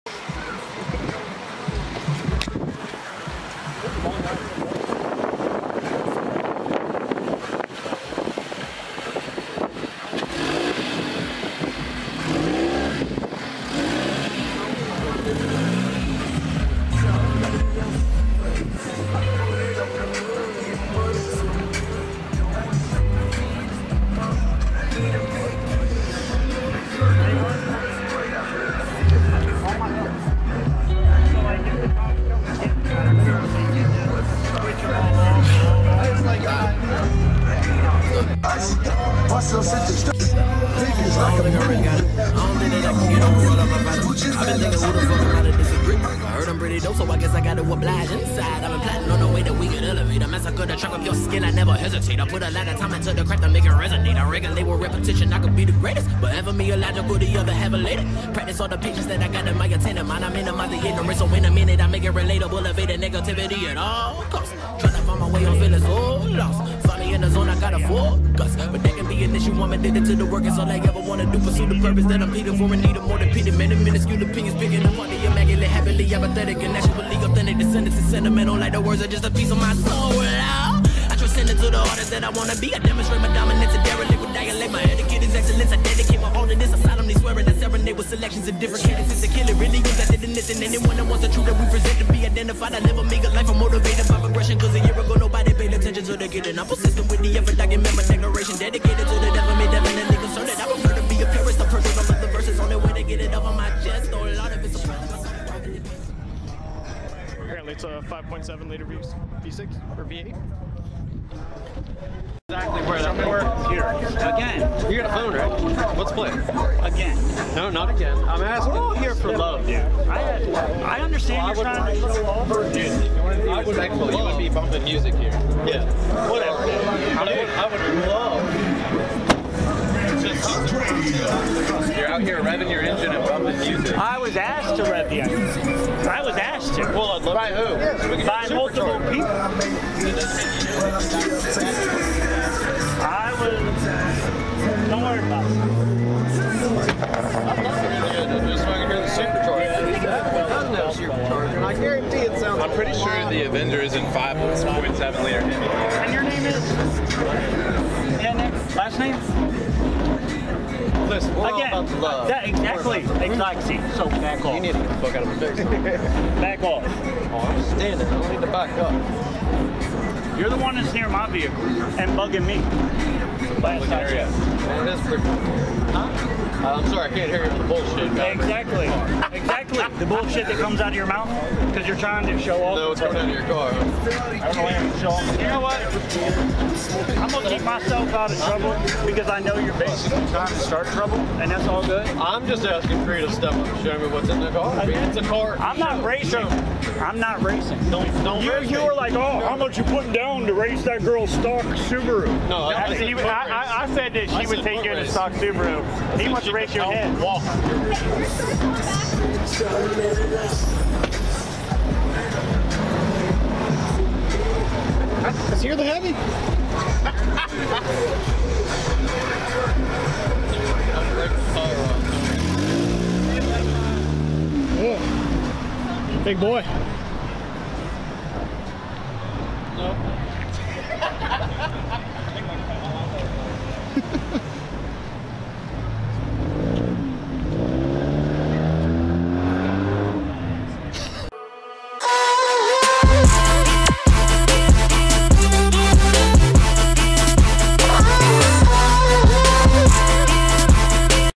CONVICTED SEX OFFENDER GETS ROASTED AT CAR MEET ("1100 hp 5.7 HELLCAT SWAPPED AVENGER")
Ricer claims his rides got 1100hp because its hellcat swapped. has fake NOS in the trunk.